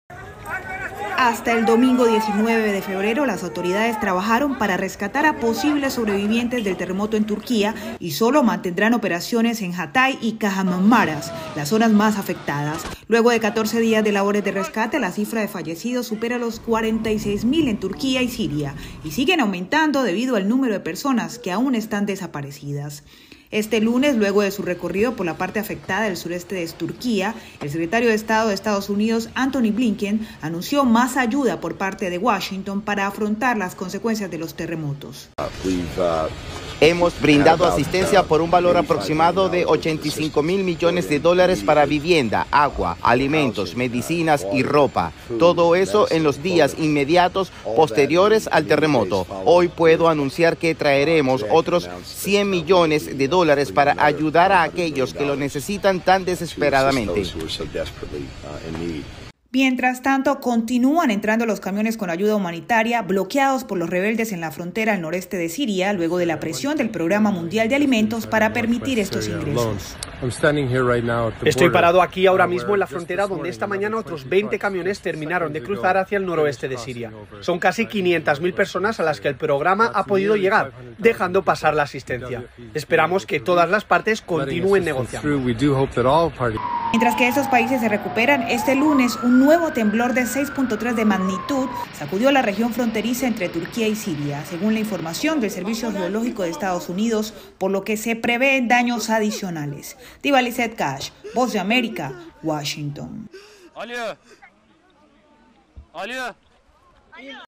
Concluyen labores de rescate por el terremoto en Turquía
Un terremoto de 6.3 grados volvió a sacudir a Turquía y Siria este lunes, cuando culminaron oficialmente las operaciones de rescate.